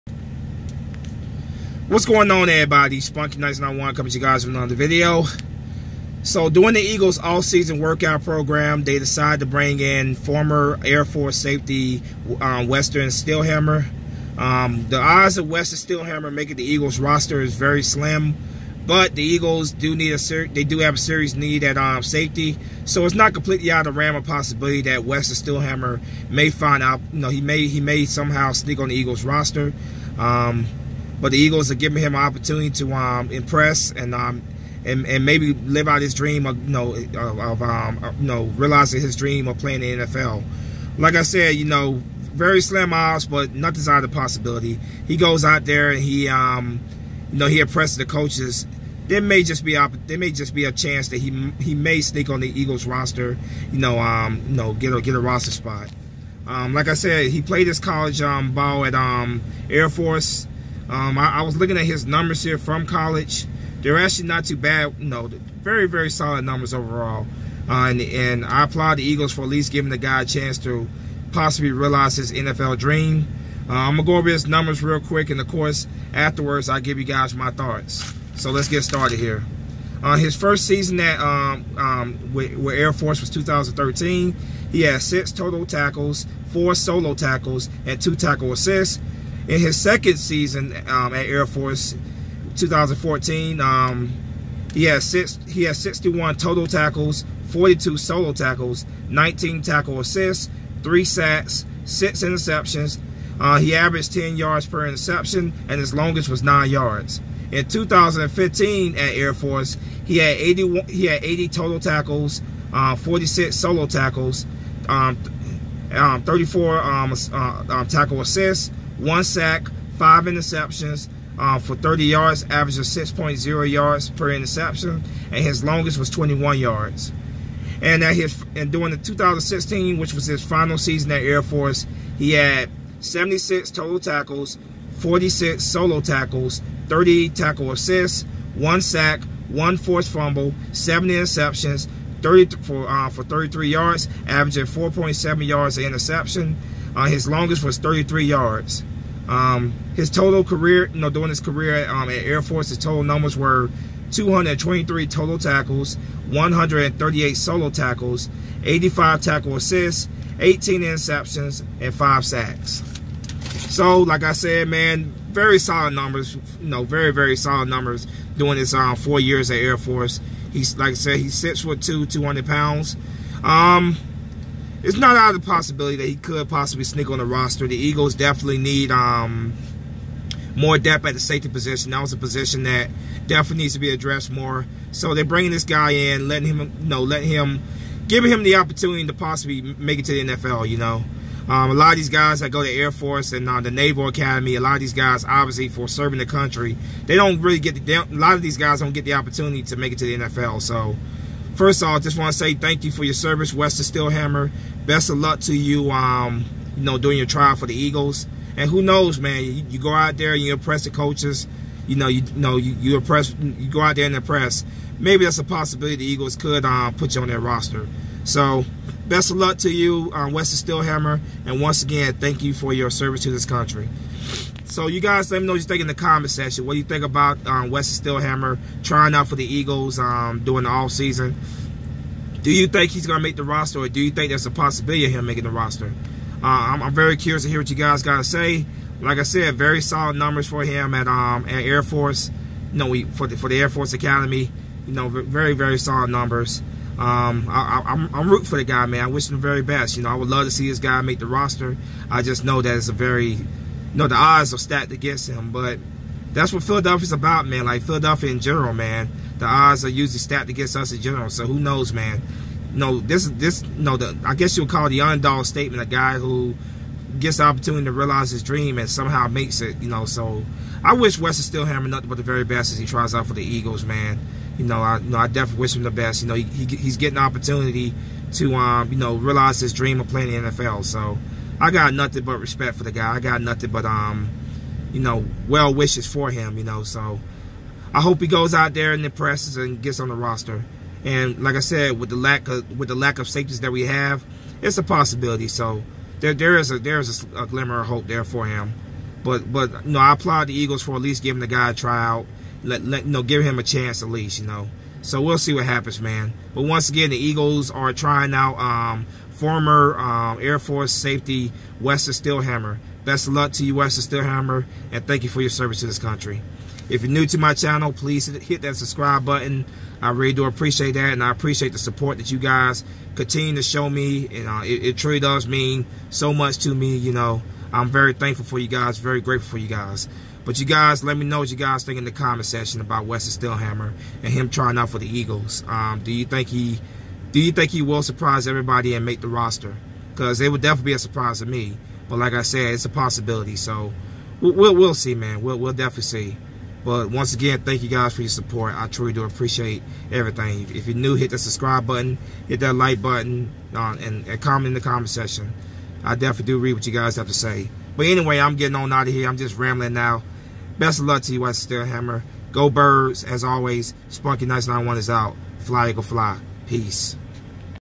Eagles tryout former Air Force safety Weston Steelhammer
Former Airforce safety Weston Steelhammer is trying out for the Eagles.